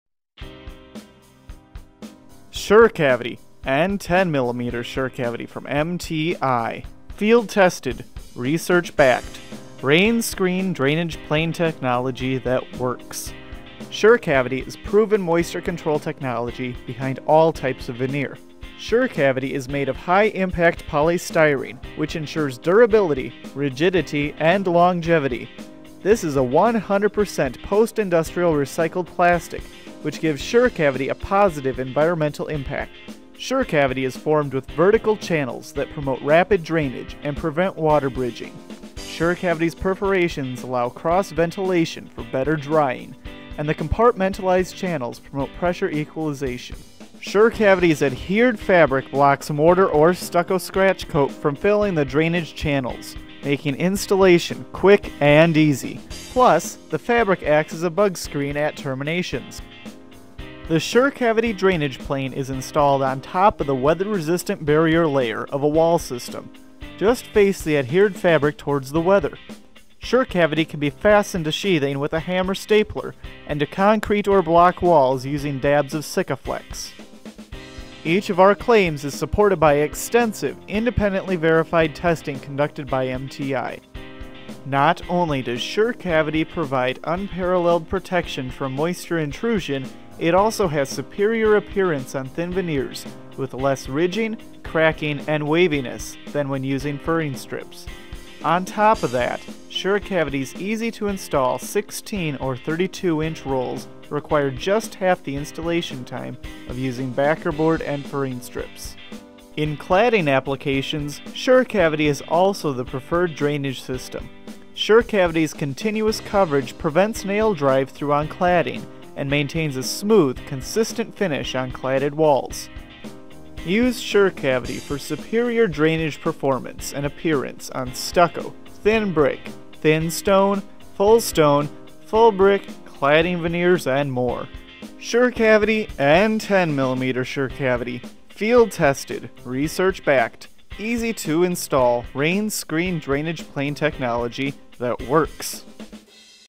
Sure Cavity Rainscreen Drainage Plane with "True Channel" Technology
Proven protection against moisture and mold problems. Prevent entrapped moisture with Sure Cavity. Sure Cavity's rigid vertical channels promote rapid drainage and won't compress under pressure. Its perforations allow cross ventilation for better drying. Sure cavity is made of 100% pre-consumer recycled plastic. Sure Cavity is easily to install and provides a superior finished look in all types of veneer.